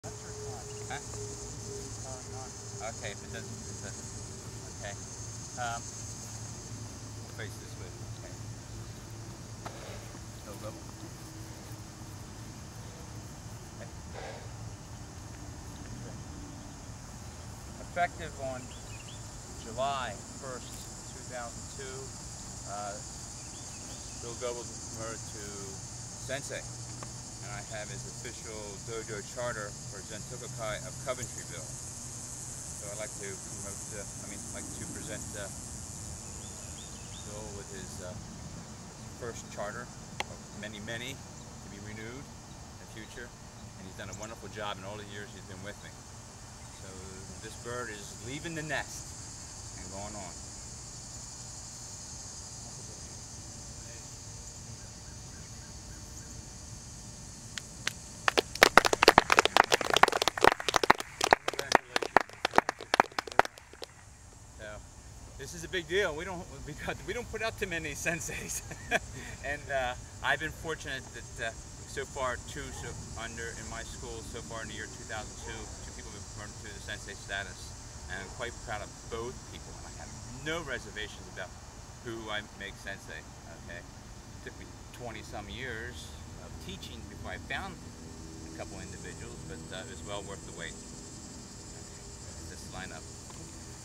Bill Goebel gets appointed the status of Sensei
Bill Goebel gets appointed with the title of Sensei by Jim Pizii Sensei in effective July 1st 2002 on Jim Pizii's property outdoors. Also Walter Dailey was present for this event.